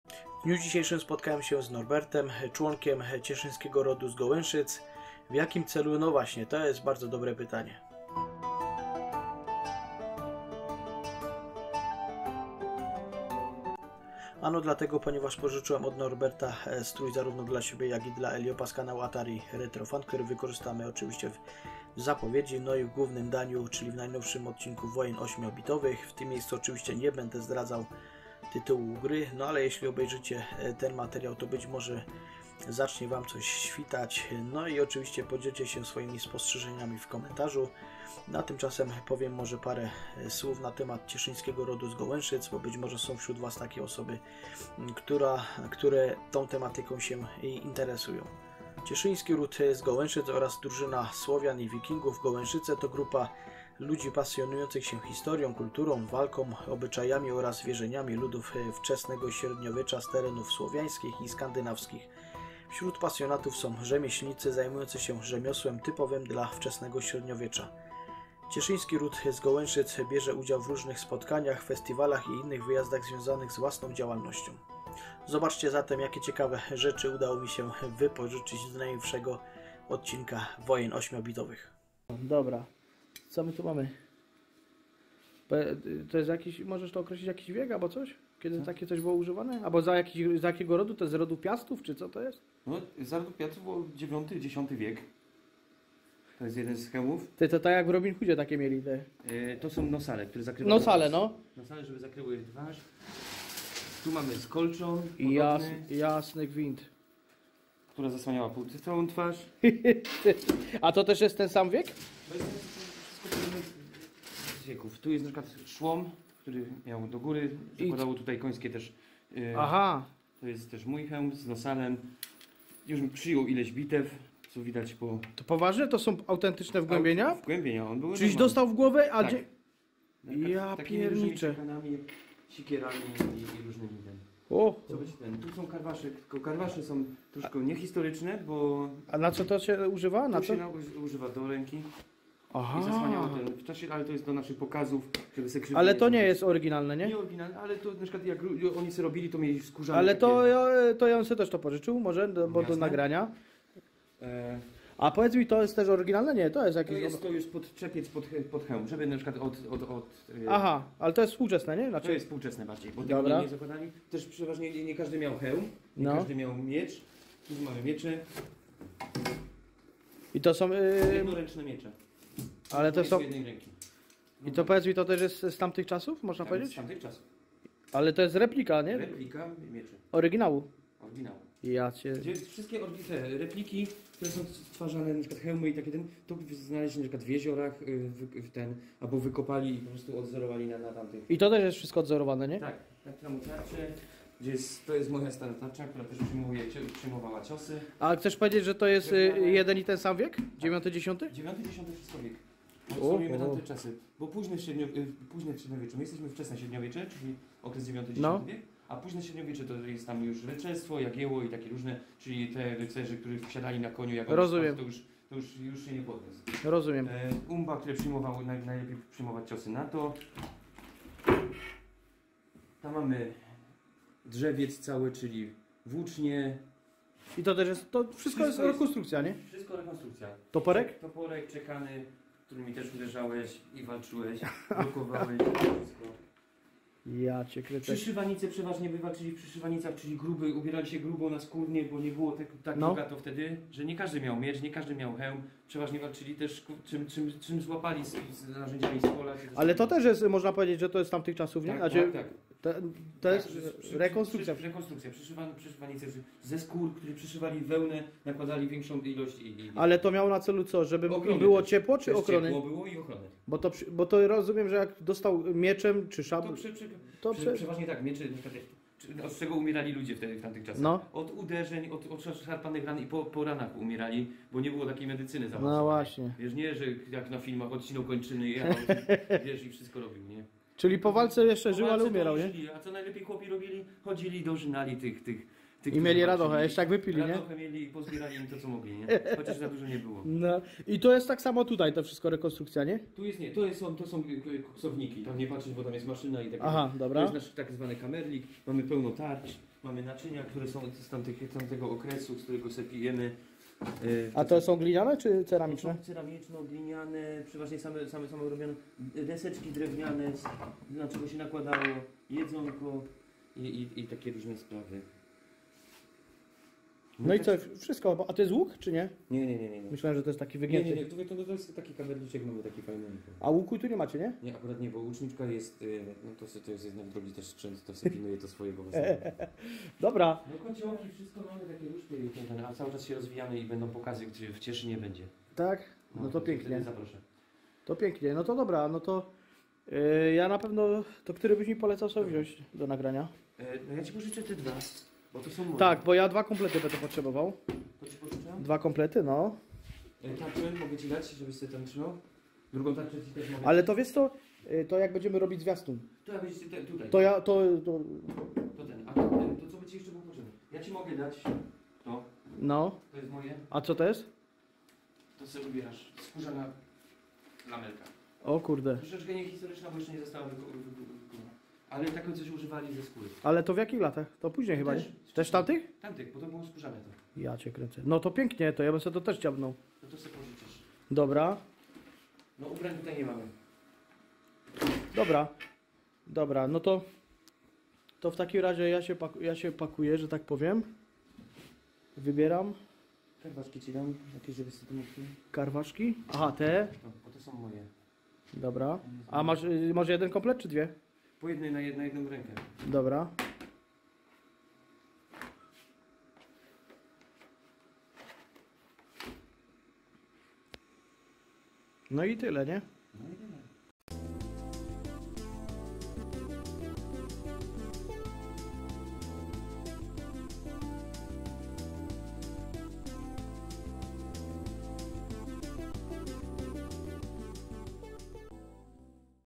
⚔ WOJNY 8 BITOWE | CIESZYŃSKI RÓD Z GOLĘSZYC - PRZYGOTOWANIA DO ODCINKA | COMMODORE 64 VS ATARI 65XE
► Trwają przygotowania do kolejnego odcinka Wojen 8 Bitowych toteż w dniu dzisiejszym spotkałem się z Norbertem - członkiem Cieszyńskiego Rodu z Gołęszyc. W jakim celu? Zobaczcie sami.

w tym miejscu zachęcamy Was do pisania i zgadywania w komentarzach jaką grę tym razem wzięliśmy na tapetę.